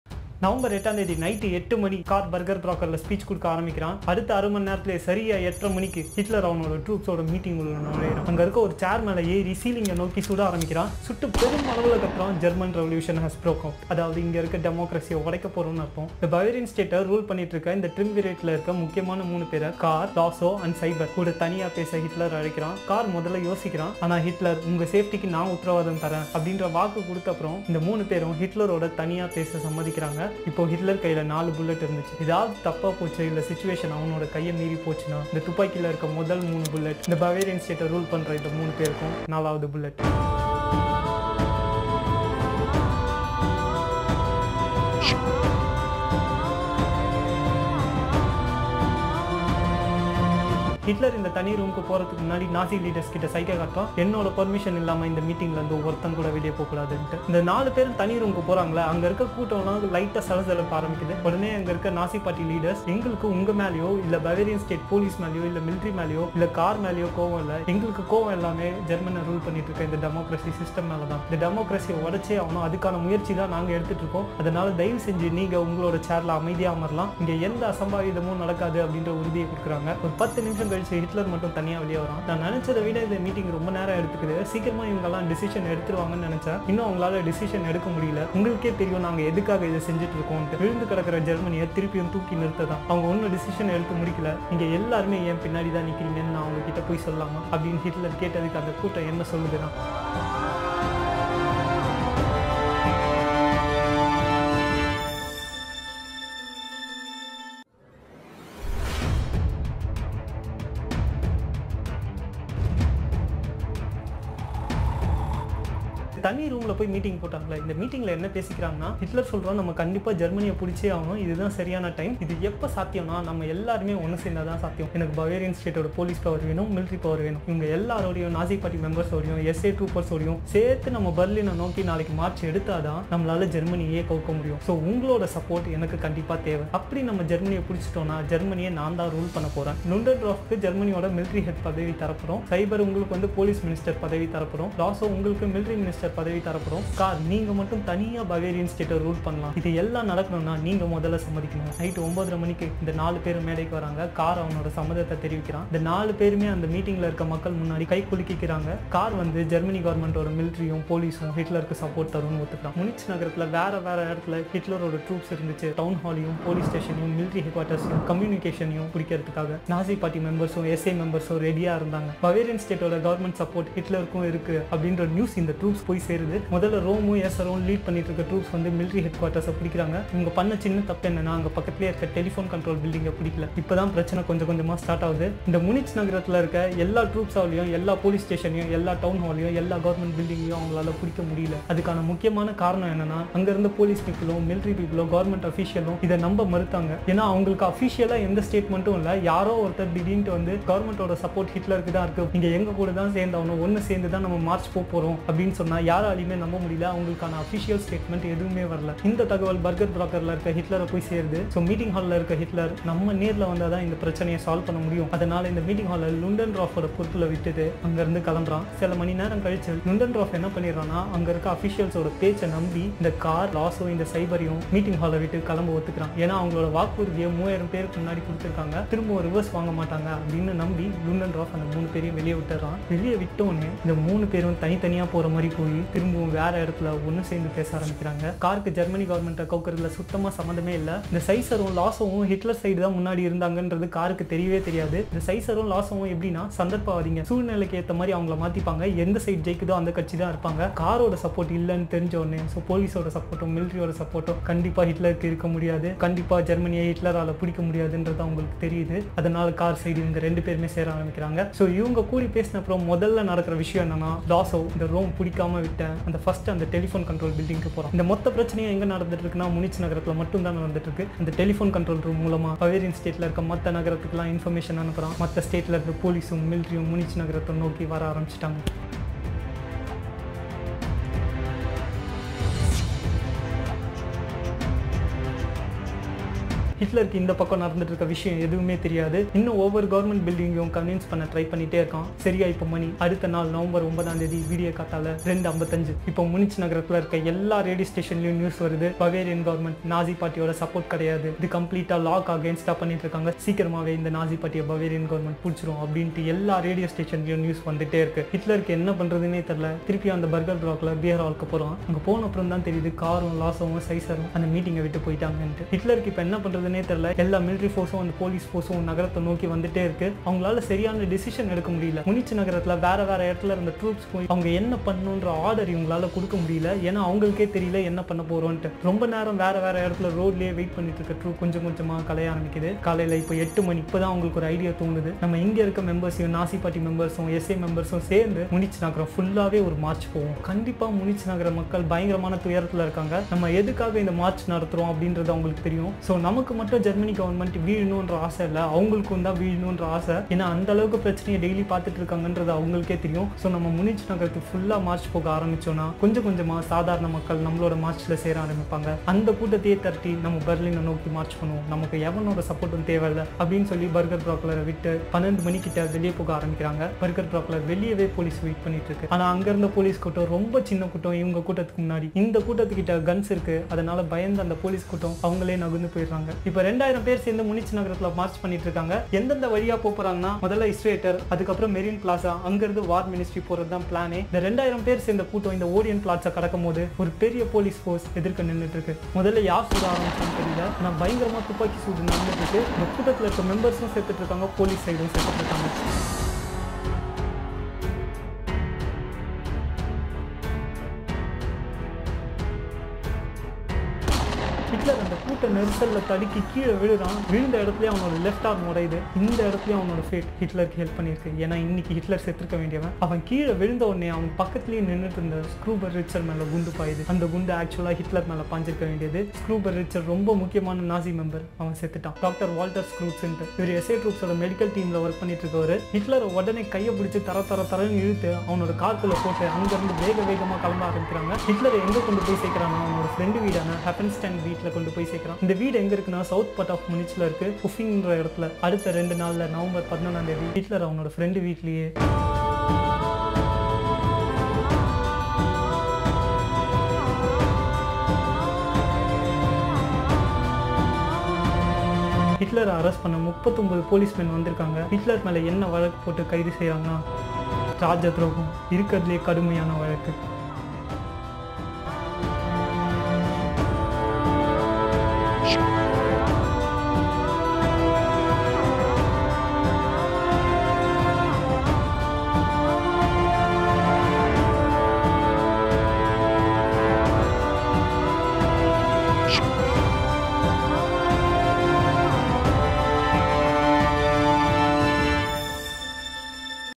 GERMAN REVOLUTION HAS BROKEN OUT | EPISODE 15 | TAMIL | COMPLETE HISTORY OF HITLER | PRADEEP KUMAR
This video is about what happened in beer hall putsch